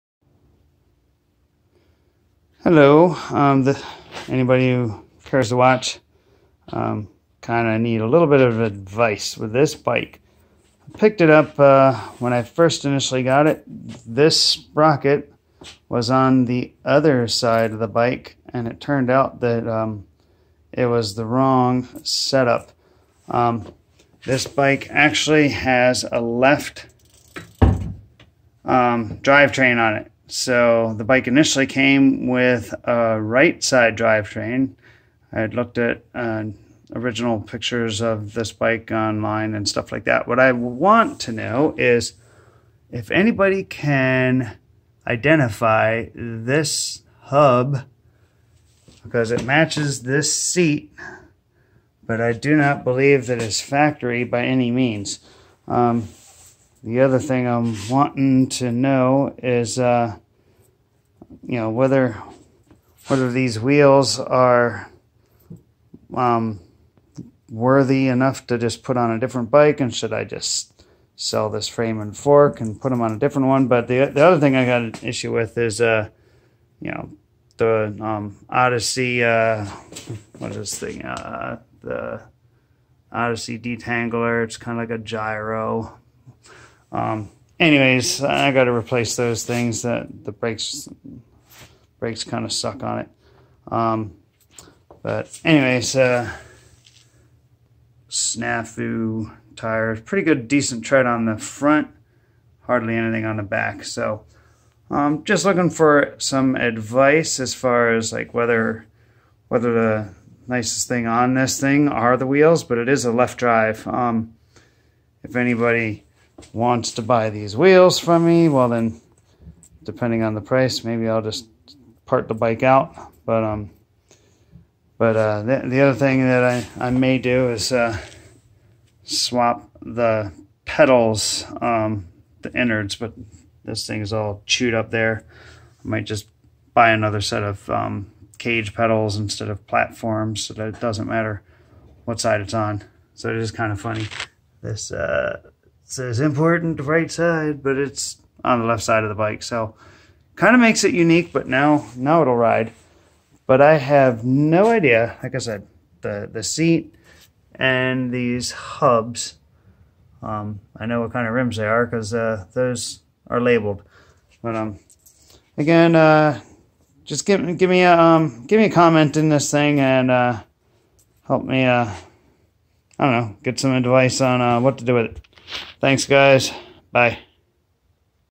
Need help identifying left side drive bmx/freestyle hubs/seat
Picked up this bike a month ago and after some time scouring the internet on Google images...I cannot seem to find what kind of hubs / seat these are.

Trying to determine whether this bike is worth more to restore with the factory wheels and to sell these? Or to put these wheels on a different bike. as well as whether to replace the brake cables or to just ride/sell with no brakes. Seems to be a trend with that. but I'm 46 years old and seem to like the security of being able to slow down without throwing my foot between the frame and the tire...